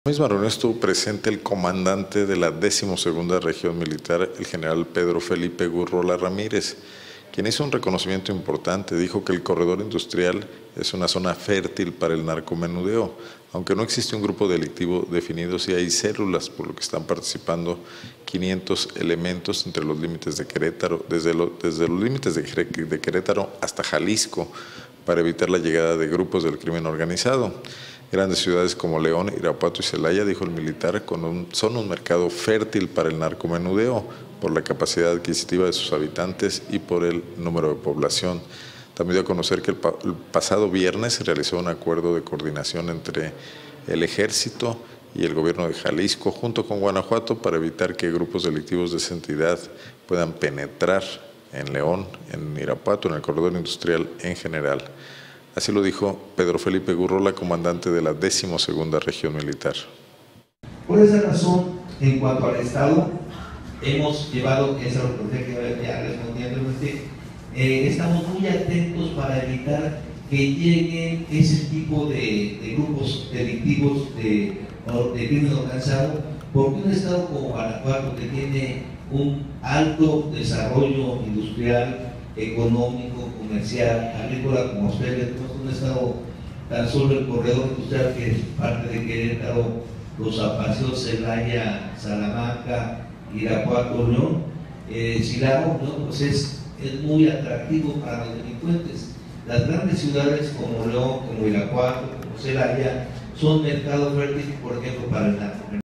‘León, Irapuato y Celaya, tierra fértil para narcomenudeo’: Pedro Felipe Gurrola
El comandante de la XII región militar, puntualizó que debido al poder adquisitivo y al número de población del corredor industrial, se da con mayor facilidad el narcomenudeo.